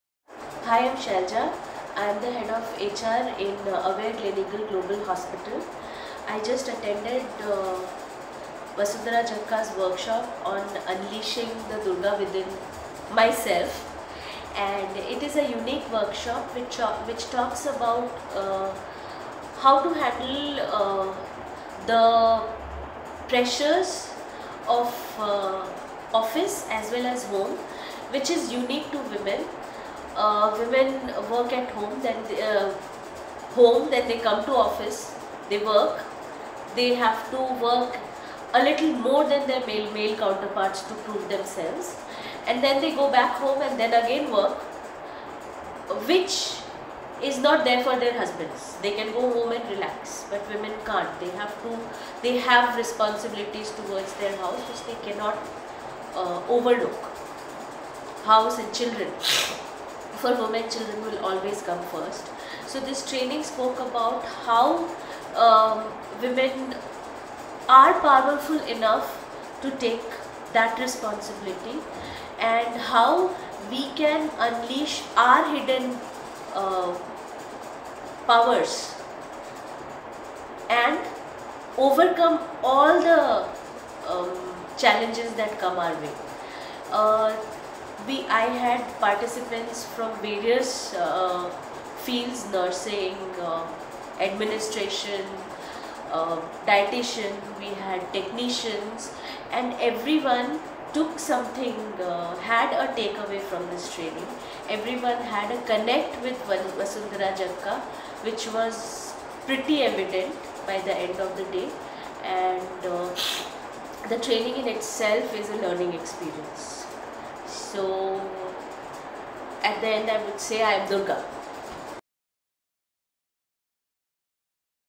Head HR-Global Hospital shares feedback on workshop DURGA
DURGA workshop for women -by Vasundhara Jakka-mind Spark Consultants